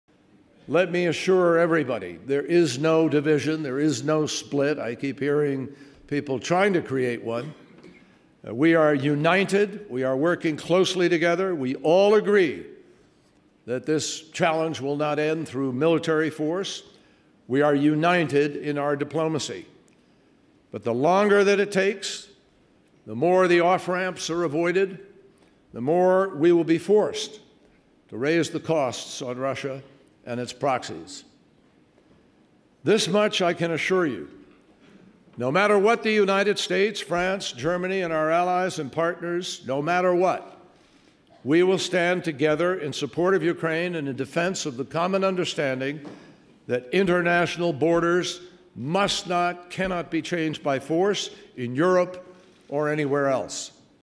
Standing Together in Support of Ukraine: “There is no division.”
U.S. Secretary Kerry at the Munich Security Conference 2015